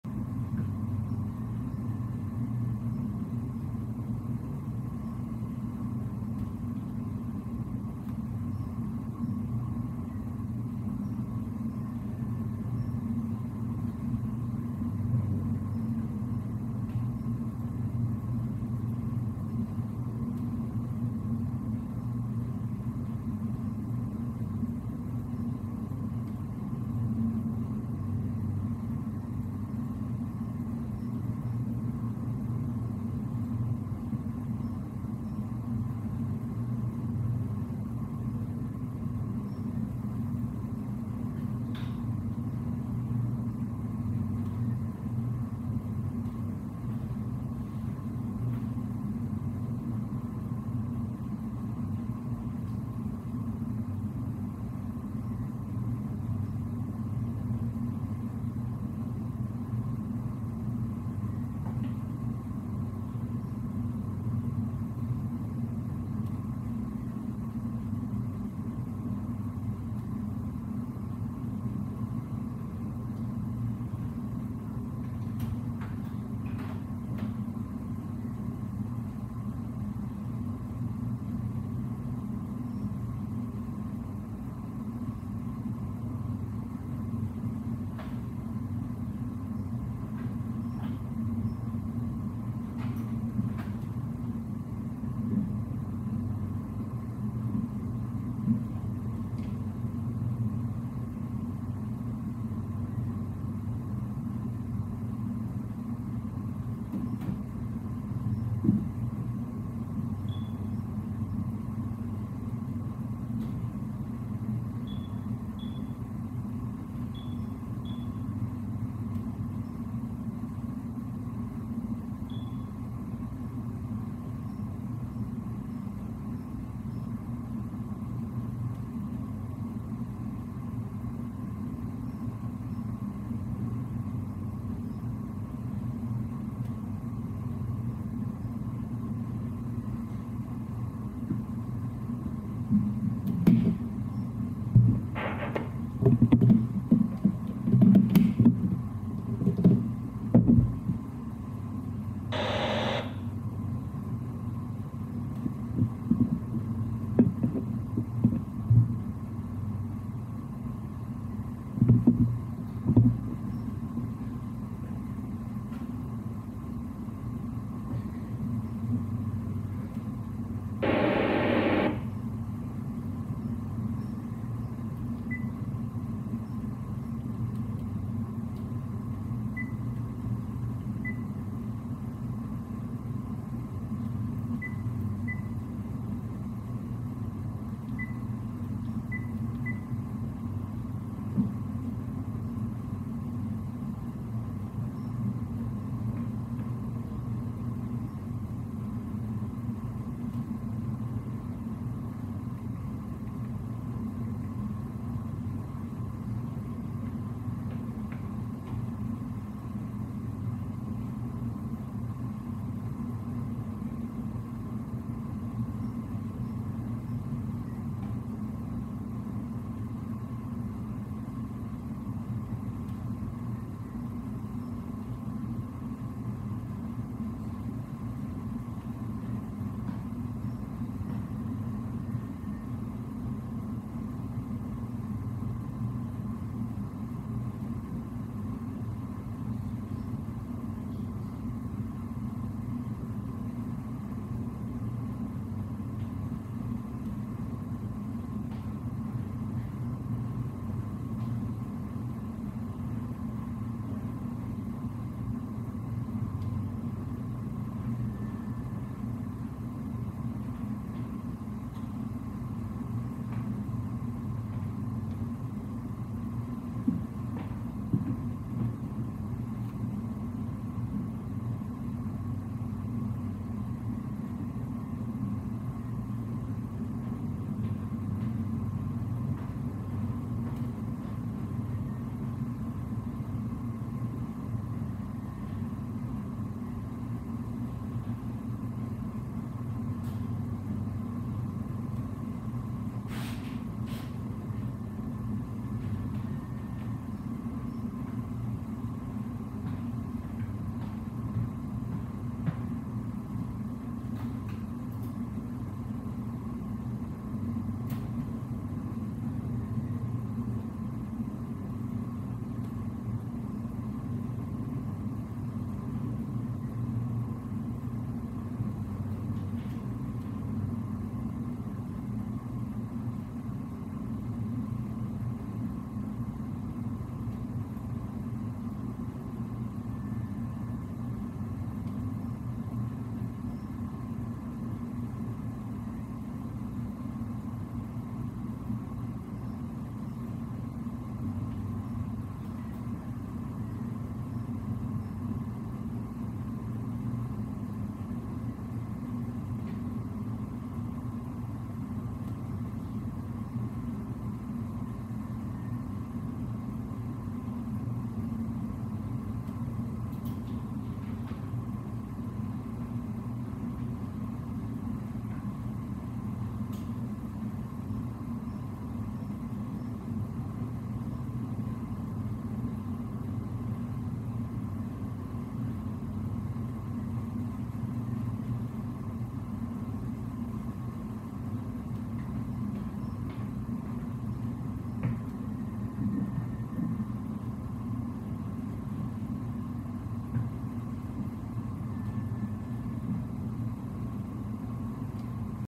Station VR, Officer bridge watch routine (항해사 선교 당직 일상)
Officer watch on bridge.
ECDIS.
RADAR.
AIS.
VHF.
NAV. CHART.
GYRO AND MAGNETIC COMPASS.
GPS.
STEERING GEAR AND AUTO PILOT SYSYEM.
ENGINE TELEGRAPH.
VDR.